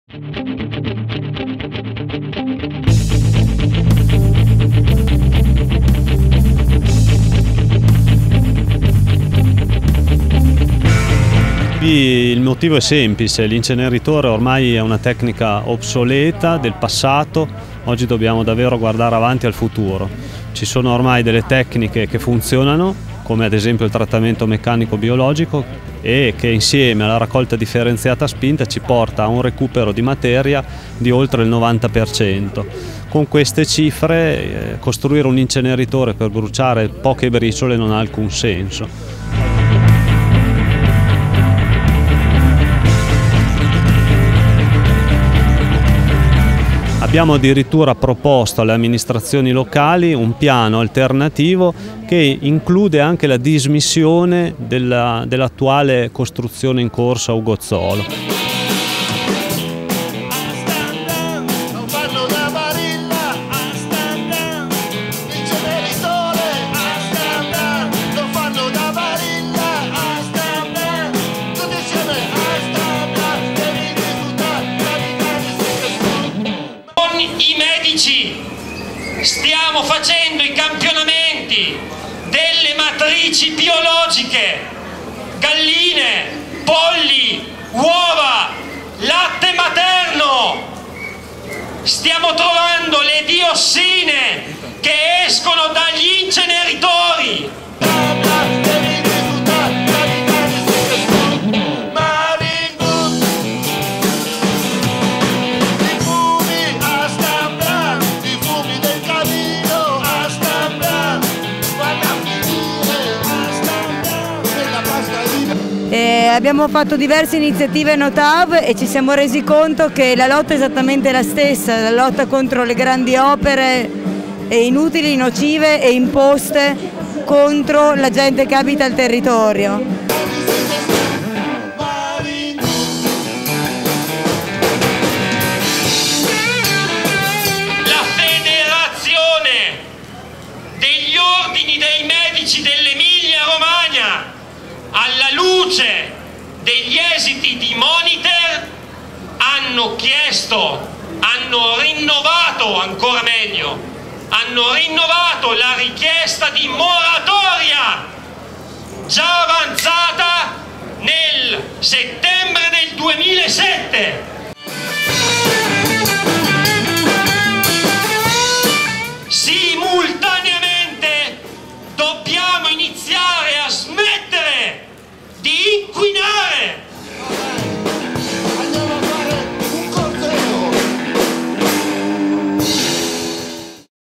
Parma: no inceneritore
14 aprile 2012, manifestazione a Parma contro la costruzione dell'inceneritore